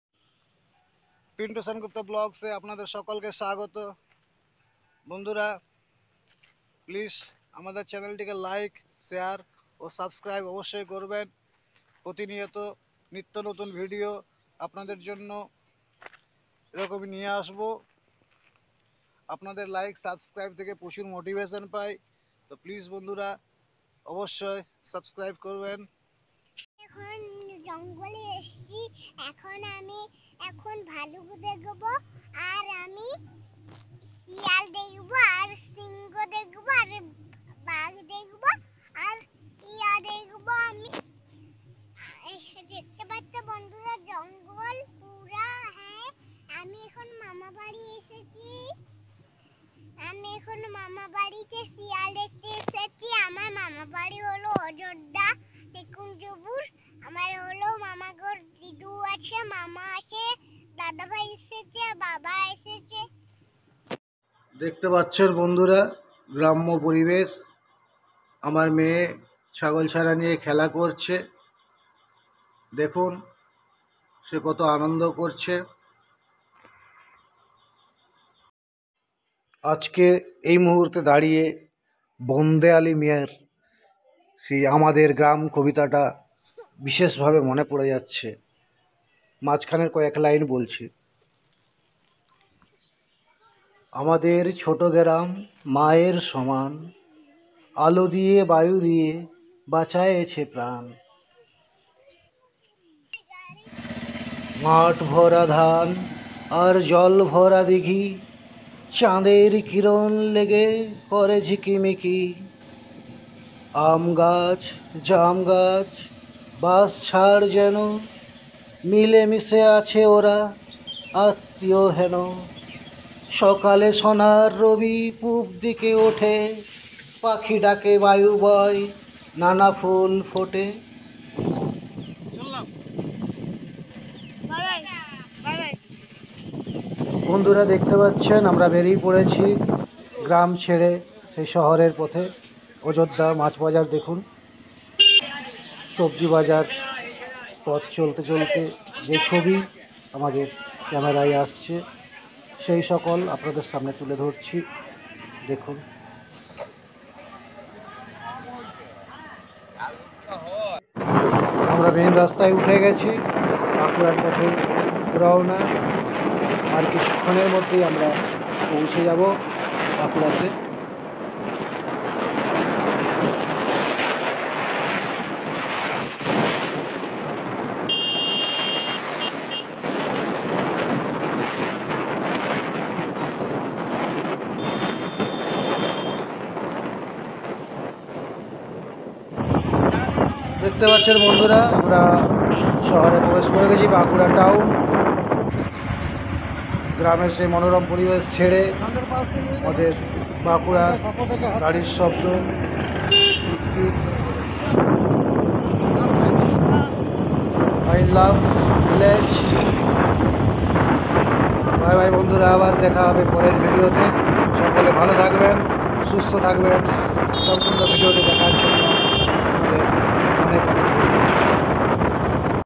I Love Village ! My Village ! Daily Vlog !pintusenguptavlog
parimalbiswas vlog#
Rimisingha vlog#
vijay riya vlog#
Rippas vlog#

I Love Village ❣️. village always a peace full, natural beauty always came from urban area people. so some natural atmosphere share with my youtube friends..